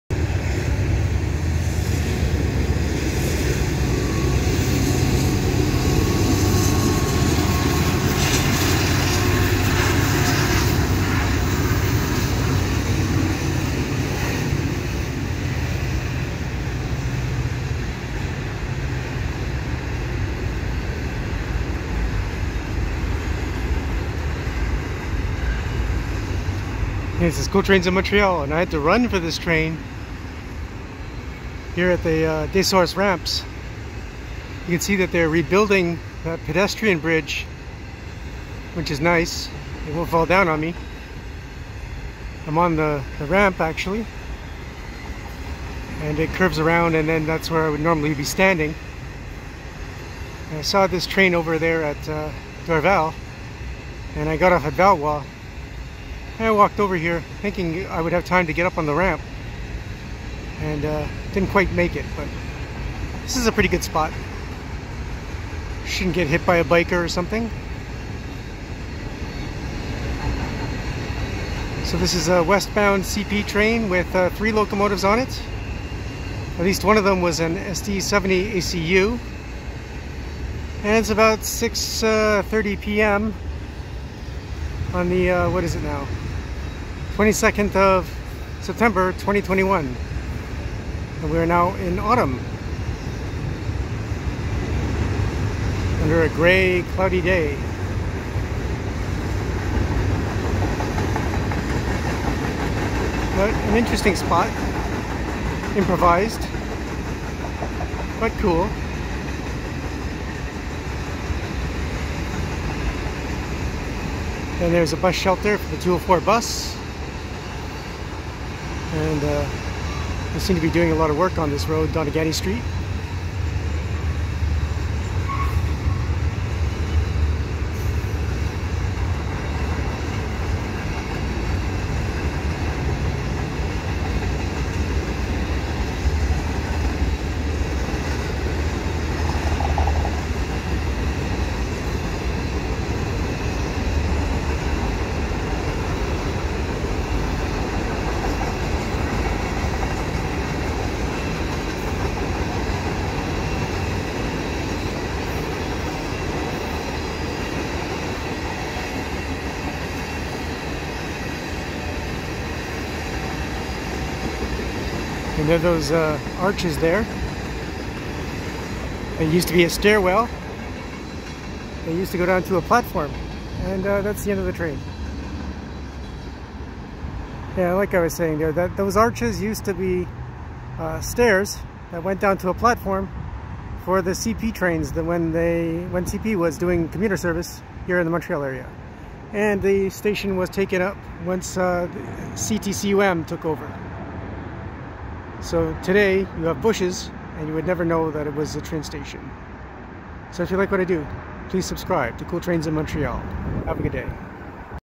TRAINS CP Freight With Three Engine at Des Sourses Ramps Bridge View
CP Westbound Intermodal with a long cut of mixed freight is headed by an AC4400CW and two EMD SD70ACU locomotives facing backwards.  The train is seen from the base of the ramp that curves to the crest of the Des Sourses Boulevard access ramps to HWY 20 on the Dorval and Points Claire  municipal border.